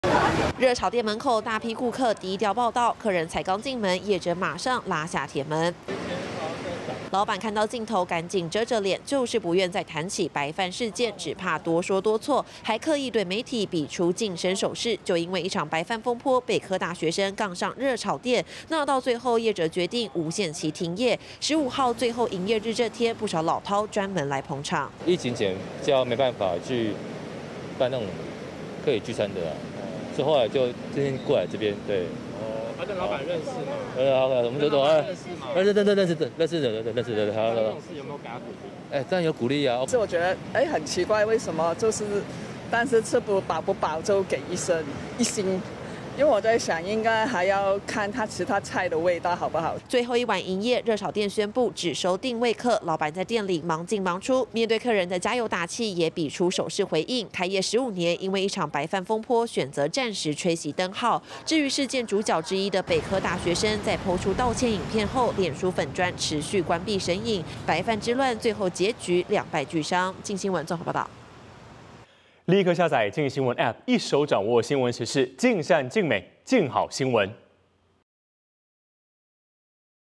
盼「白飯之亂」落幕！熱炒店最後一夜 老闆：訂位全滿｜鏡新聞
「白飯之亂」可能在今日（7/15）正式畫下句點嗎？ 北科大學生和熱炒店，因為白飯引發糾紛，事件連日來在網路上發酵，業者最終宣布從7/16起暫停營業，今日（7/15）晚上最後一晚營業，老闆雖低調不受訪，但仍透露訂位數全滿，有不少客人來給他加油打氣。